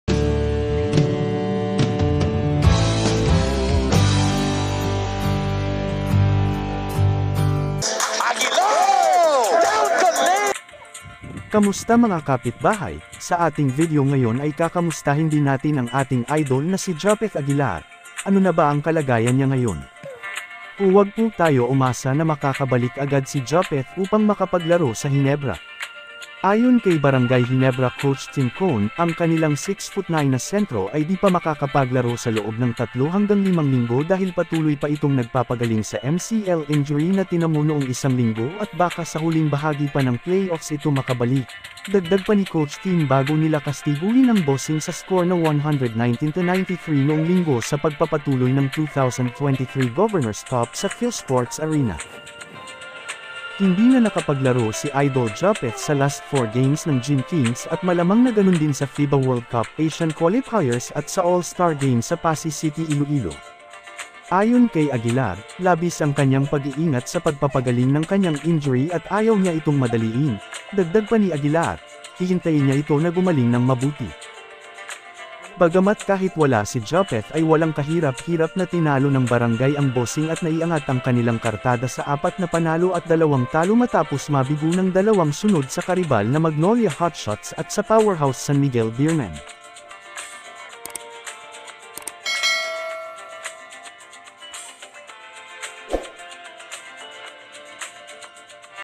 JAPETH OUT NA BA SA GILAS AT ALL STAR GAMES?
Japeth out for 3 to 5 Weeks